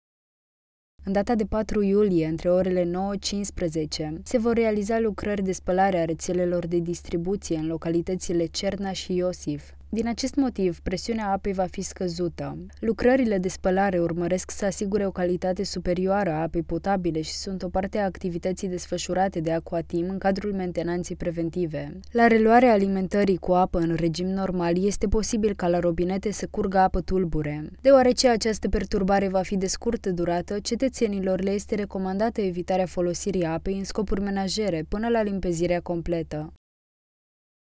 Presiunea apei scăzută în mai multe localități
În data de 4 iulie, între orele 09:00-15:00, se vor realiza lucrări de spălare a rețelelor de distribuție în localitățile Cerna și Iosif. Din acest motiv, presiunea apei va fi scăzută. Lucrările de spălare urmăresc să asiguirea o calitate superioară a apei potabile și sunt o parte a activității desfășurate de Aquatim în cadrul mentenanței preventive. La reluarea alimentării cu apă în regim normal este posibil ca la robinete să curgă apă tulbure